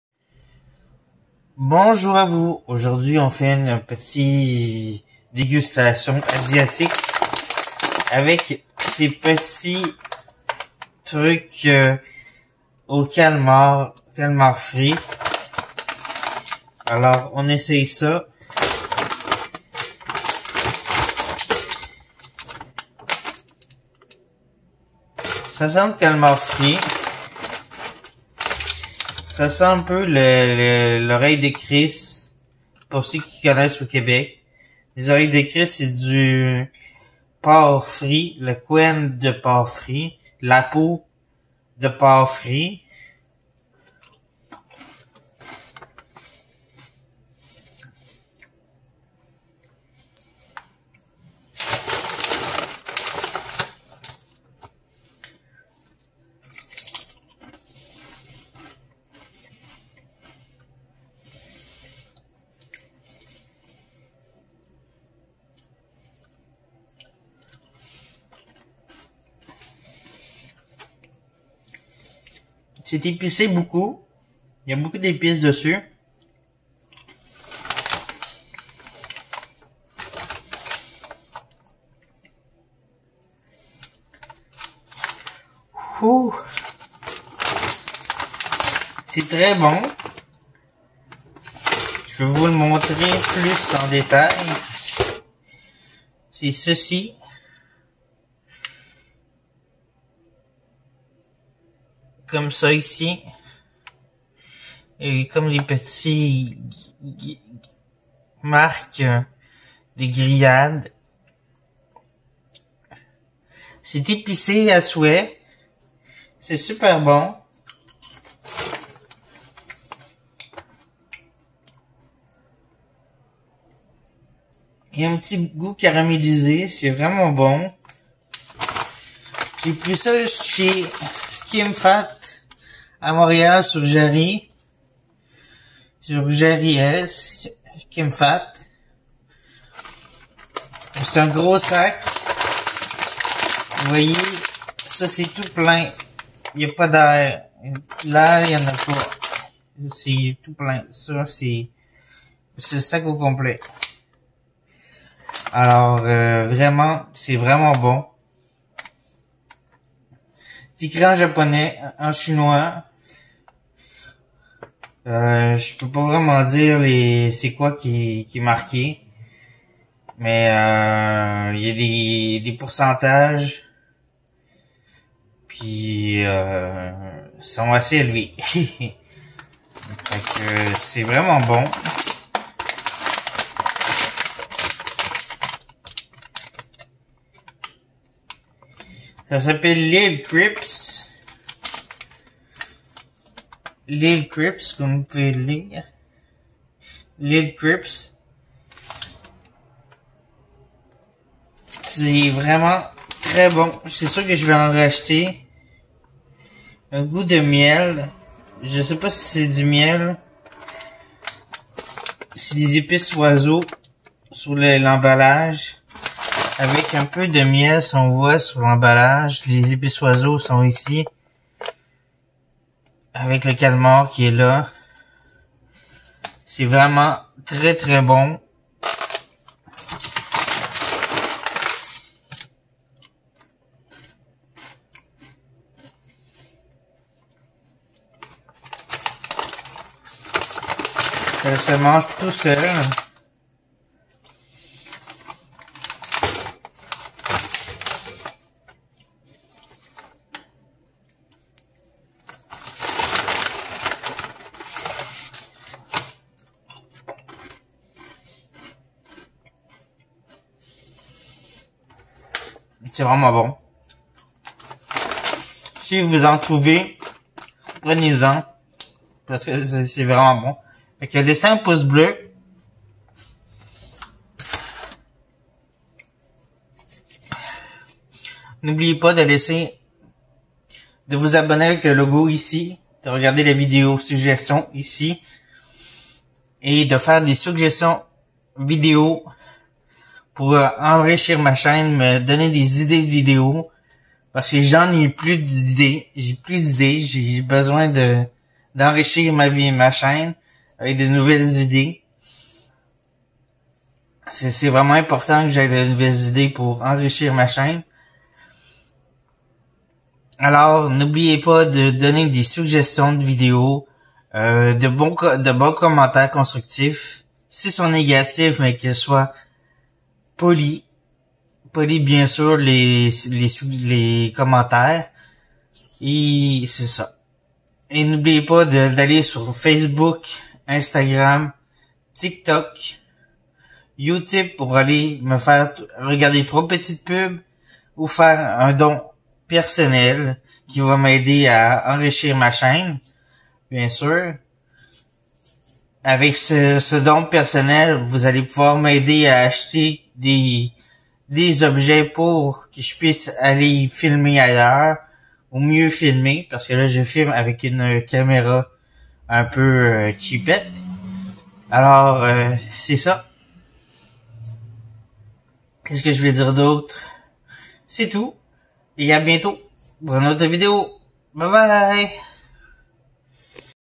JE TEST LES CROUSTILLE DE CALEMAR FRIT AU PIMENT OISEAUX ET MIEL...
je teste des croustille de calemar